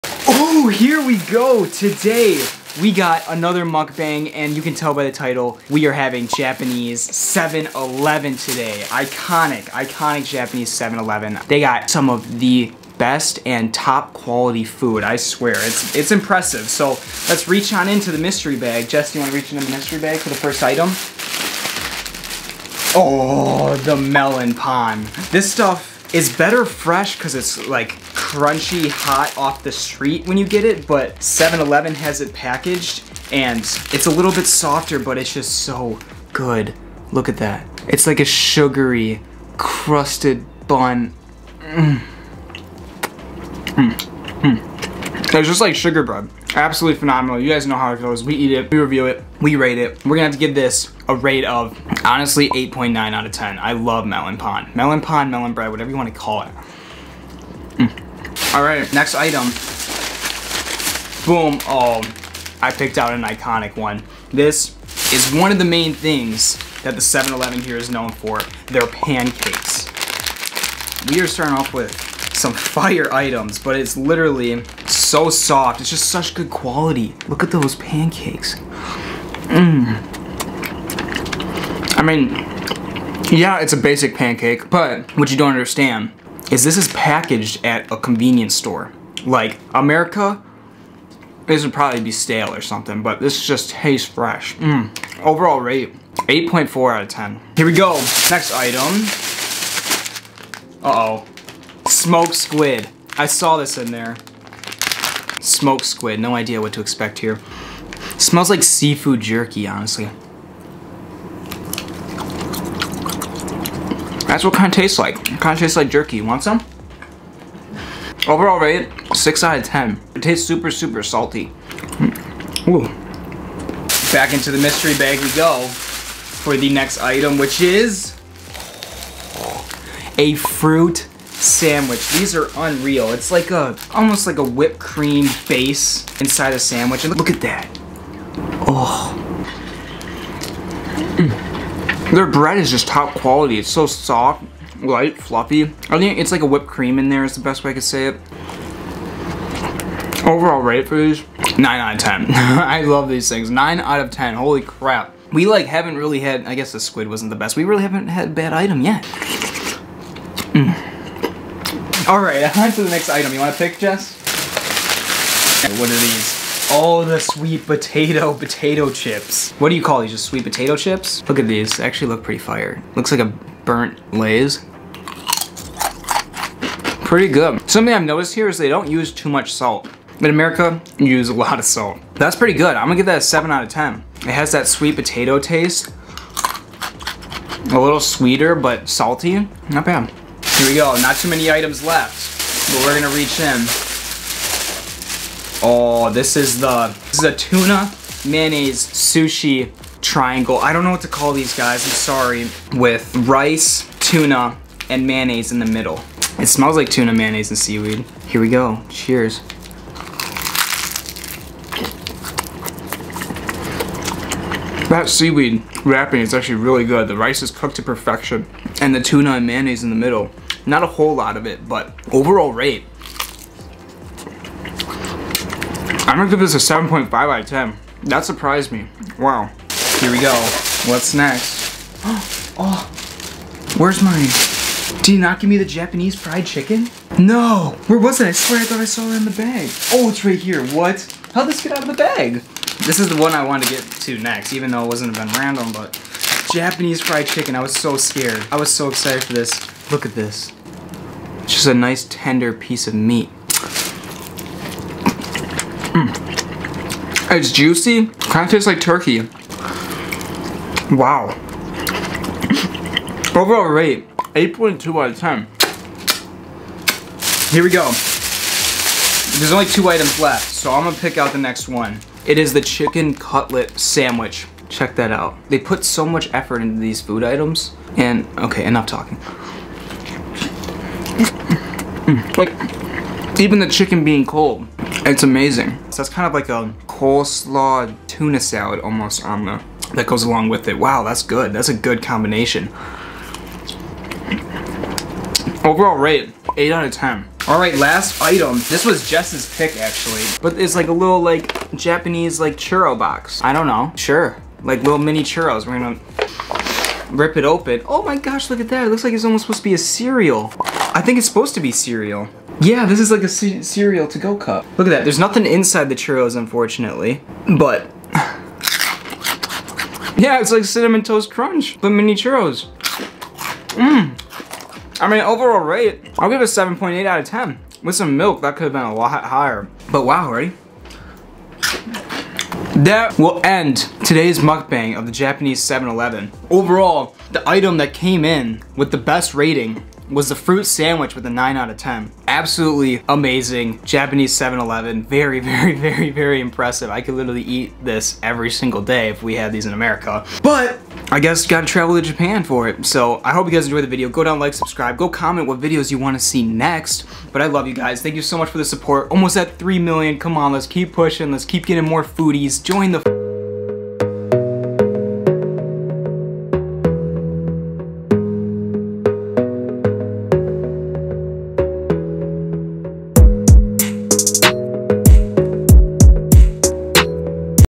EATING JAPANESE 7-ELEVEN • MukBang
Welcome to my page where we traveling everywhere and eat everything! Today is a special one because we are all the way in Japan eating from their famous 7-Eleven where they have some of the best foods you have ever seen from a convenient store.